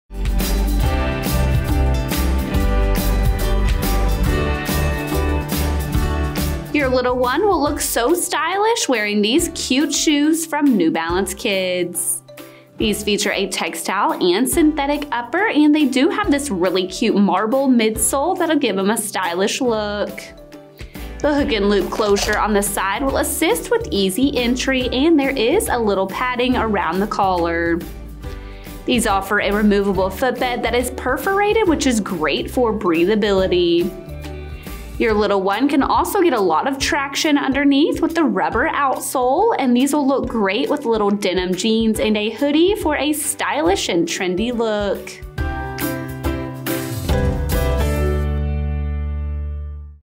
New Balance Kids 515 (Infant/Toddler) SKU: 9614893
The  New Balance&reg; Kids 515 shoes are going to be your little enthusiast's all time favorite.
Breathable synthetic mesh lining and padded textile insole provide additional stability.
Sewn logo patch with iconic brand marking along the tongue.
Adjustable hook-and-loop closure.
Gripped rubber outsole with traction lugs.
Imported.